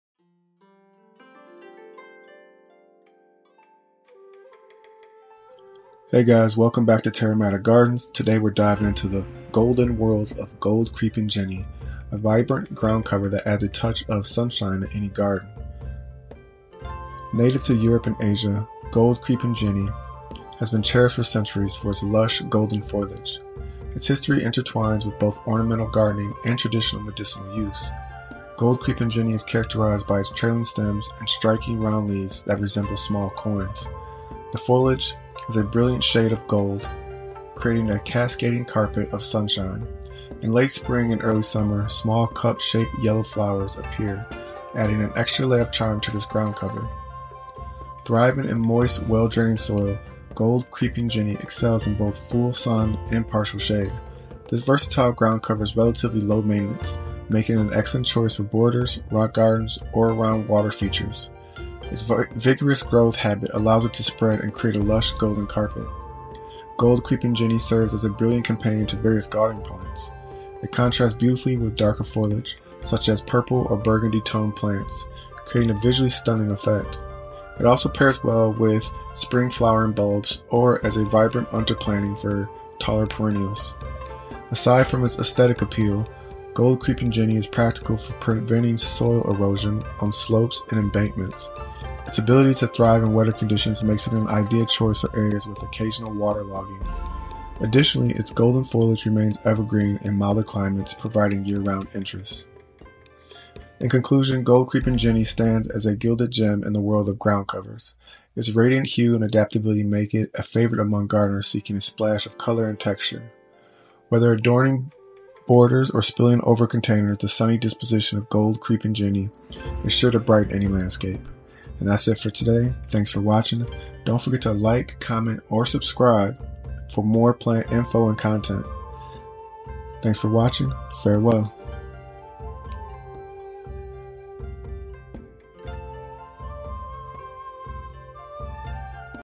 GOLD CREEPING JENNY - Educational Dive into the Lush Ground Cover of (Lysimachia nummularia 'Aurea')
Dive into an educational journey exploring GOLD CREEPING JENNY, the lush ground cover of Lysimachia nummularia 'Aurea.' 🌿 This video provides insights into the horticultural wonders of Gold Creeping Jenny, celebrated for its vibrant golden foliage and cascading growth. Whether you're a gardening enthusiast or a learner in horticulture, this exploration of GOLD CREEPING JENNY is an educational delight you won't want to miss!

Latin name: Lysimachia nummularia 'Aurea'
Common name: Gold Creeping Jenny, Moneywort
Type: Perennial ground cover
Sun Exposure: Full sun to partial shade
Soil Requirements: Moist, well-draining soil
Watering: Keep soil consistently moist, especially in hot weather
Height: 2 to 4 inches
Spread: Spreads and trails, forming a dense mat
Spacing: Plant 12 to 18 inches apart
Foliage: Rounded, golden-yellow leaves
Bloom Time: Late spring to early summer; small, cup-shaped yellow flowers
Propagation: Easily propagated from cuttings or by division
Uses: Ground cover, trailing over walls or in hanging baskets
Maintenance: Trim to control growth and maintain shape; can be aggressive in ideal conditions
Special Features: Bright golden foliage adds vibrant color; suitable for both ground cover and container planting
Hardiness Zones: 3 to 9
Note: Gold Creeping Jenny is valued for its low-growing, trailing habit and vibrant golden foliage, making it a popular choice for adding color and texture to various garden settings.